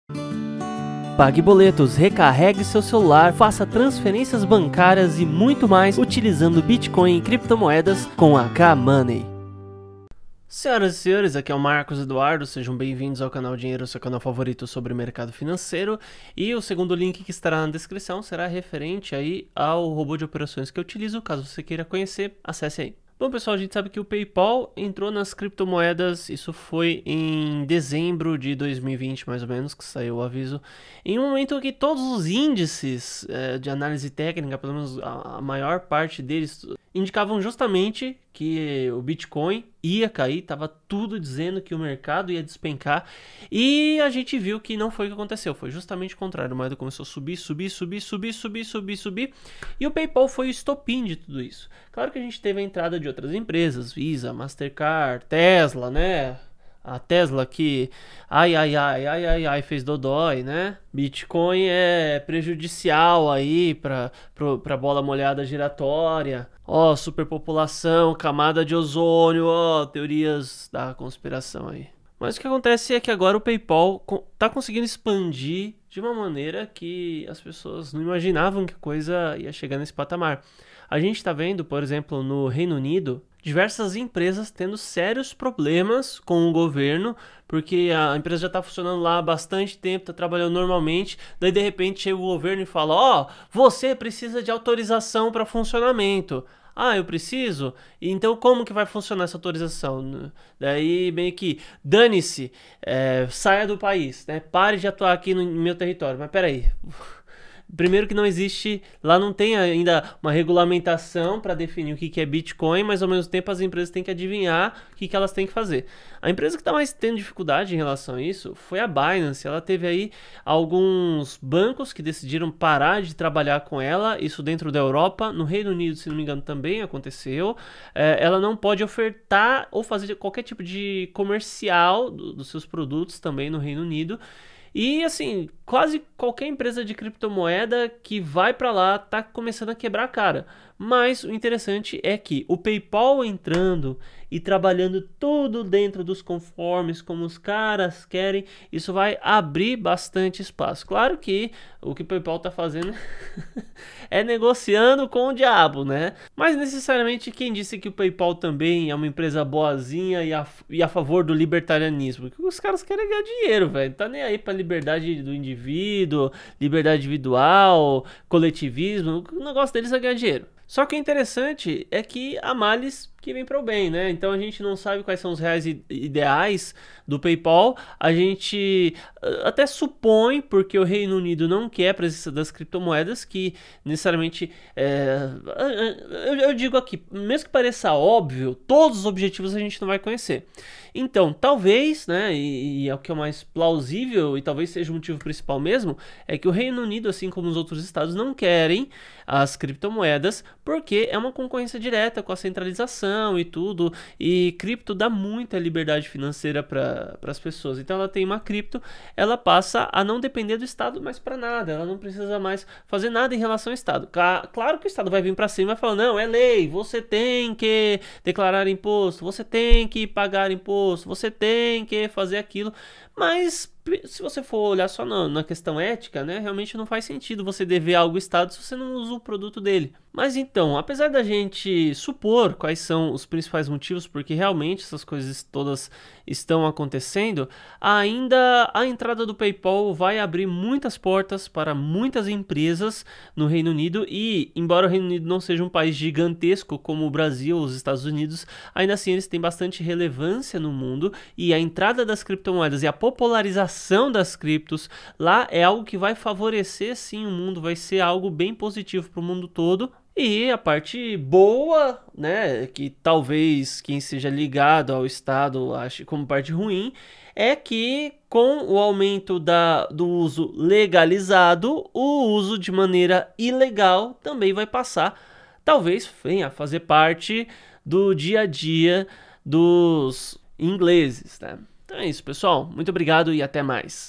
PAYPAL VAI ACEITAR MAIS CRIPTOMOEDAS? QUAIS SERÃO?
PayPal, que é uma das maiores empresas de pagamento do mundo, levou muitos clientes a entrarem nas criptomoedas através de seus serviços especializados. E agora planejam expandir suas fronteiras.